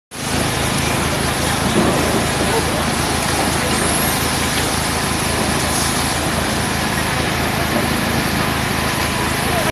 Corpo de Bombeiros combate chamas em caminhão em Mossoró, RN
O Corpo de Bombeiros Militar do Rio Grande do Norte (CBMRN) foi acionado, na manhã deste sábado (5), para combater as chamas em um caminhão na Avenida Alberto Maranhão, na cidade de Mossoró no Rio Grande do Norte.
O fogo começou por volta das 11h e os bombeiros precisaram de cerca de 20 minutos para debelar totalmente as chamas.
Ninguém se feriu. A causa para o incêndio ainda serão esclarecidas.
Cezar Alves 🎥